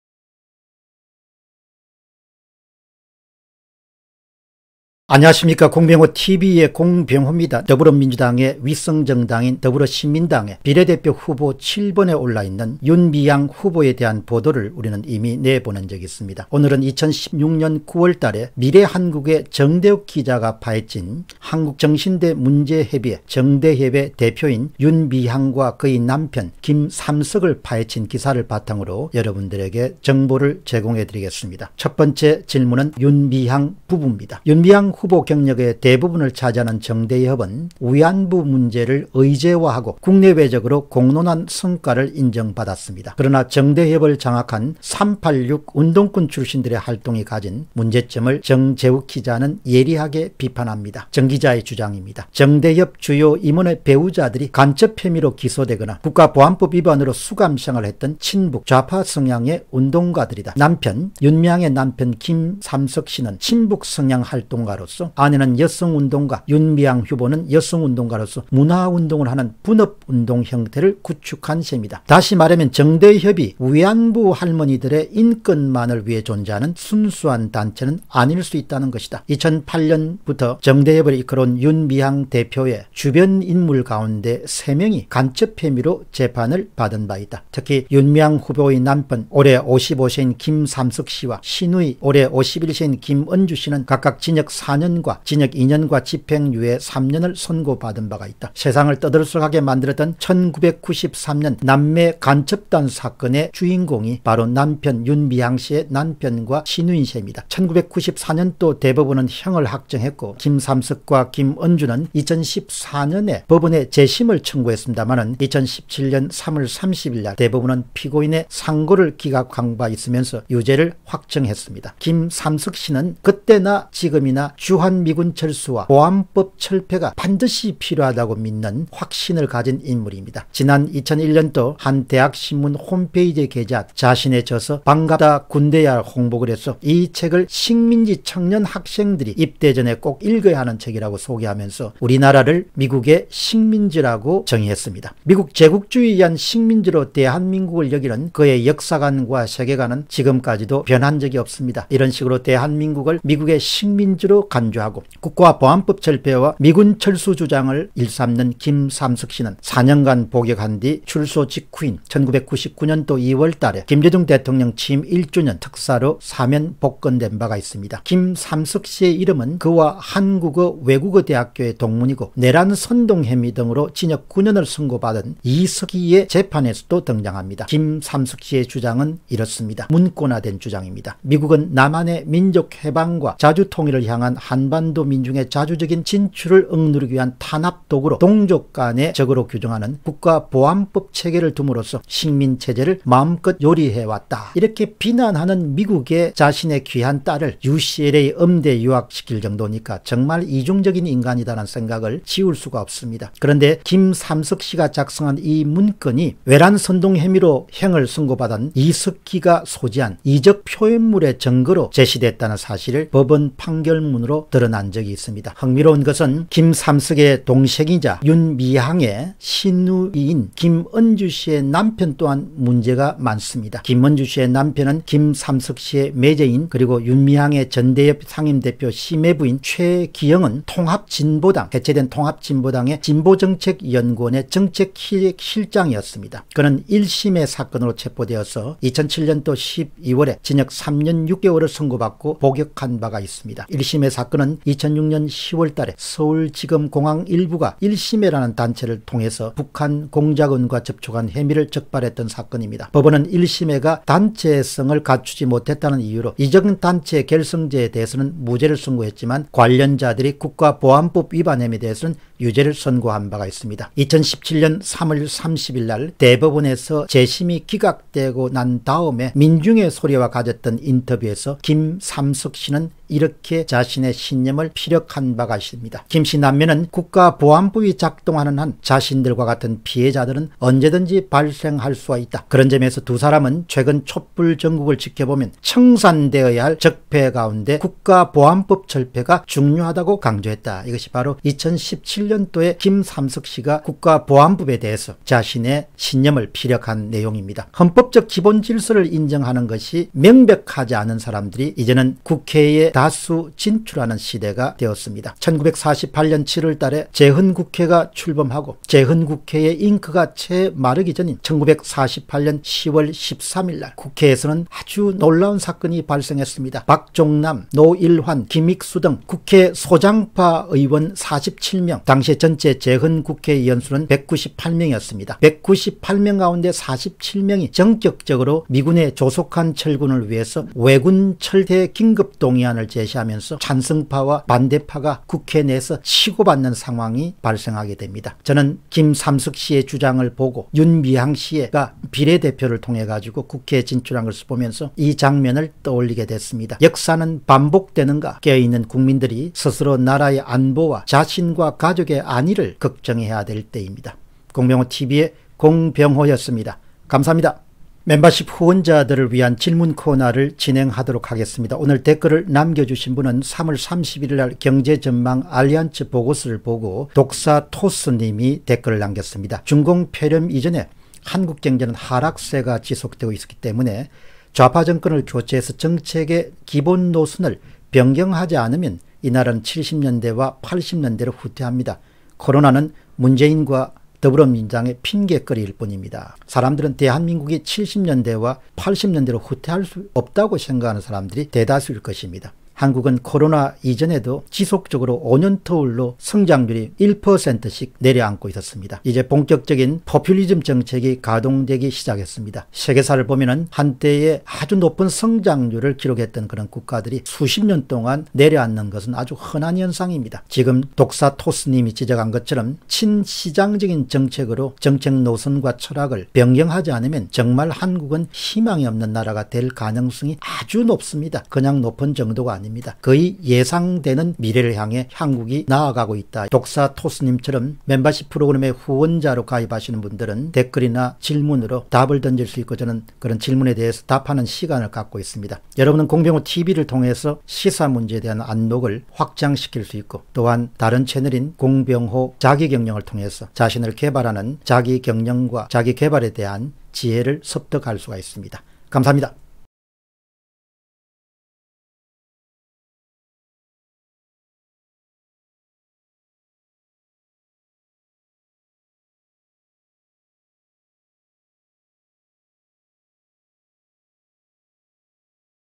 윤미향 부부 반미운동 / 더불어시민당 비례대표 7번 [공병호TV]
윤미향 부부 반미운동 / 더불어시민당 비례대표 7번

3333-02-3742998 카카오은행 (예금주 : 공병호) / (텔레뱅킹 : 090 & 은행코드 : 90)

참고자료
1. 반미 앞장서온 시민당 윤미향, 정작 딸은 미국 유학중, 김은중, 조선일보, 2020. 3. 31
2. 정대협을 움직이는 사람들, 정재욱, 미래한국, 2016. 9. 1
3. [인터뷰] 20년 넘게 ‘간첩’ 딱지 못 뗀 ‘안기부 조작’ 피해자들, 박소영, 민중의 소리, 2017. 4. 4